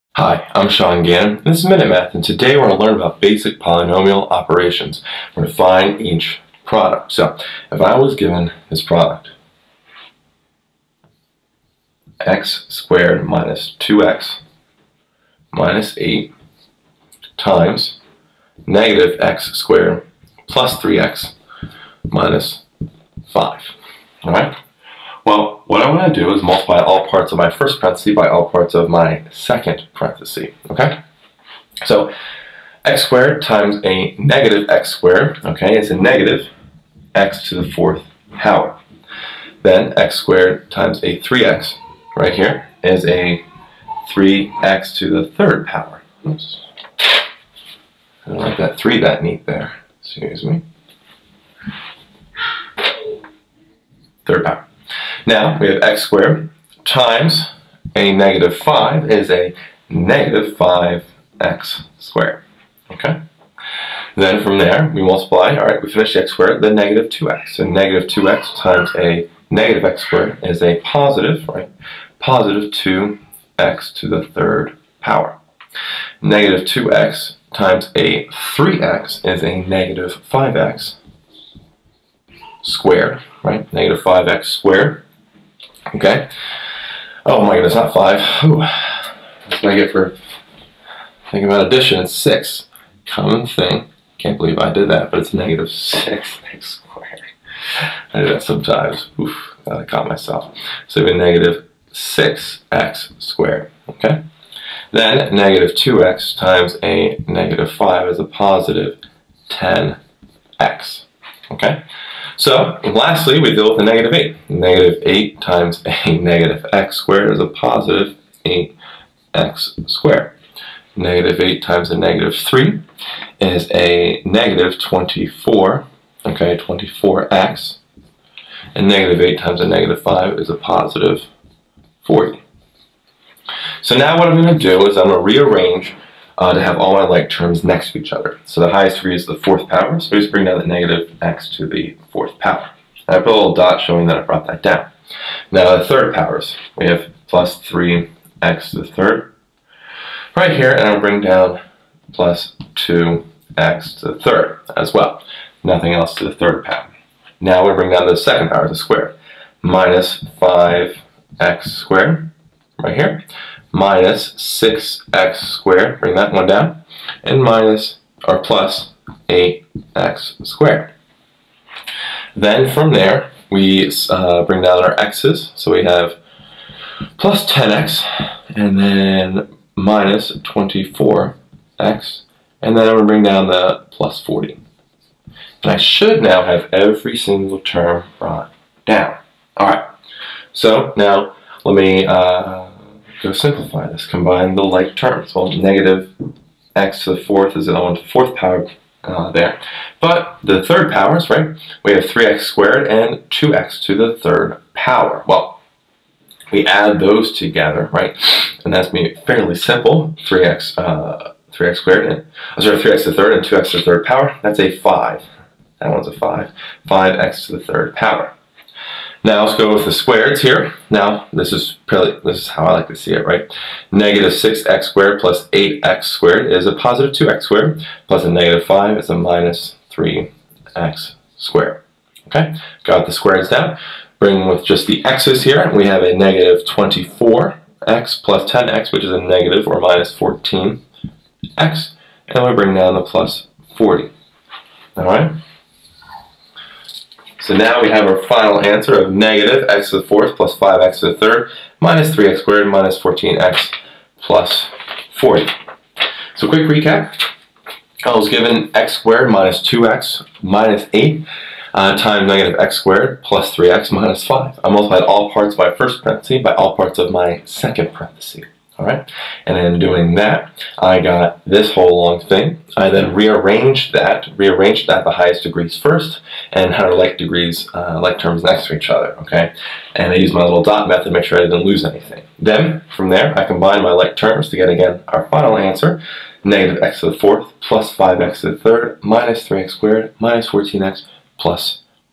Find the Product (x^2-2x-8)(-x^2+3x-5)
A math video lesson on Basic Polynomial Operations. In this video we find the product of (x^2-2x-8)(-x^2+3x-5)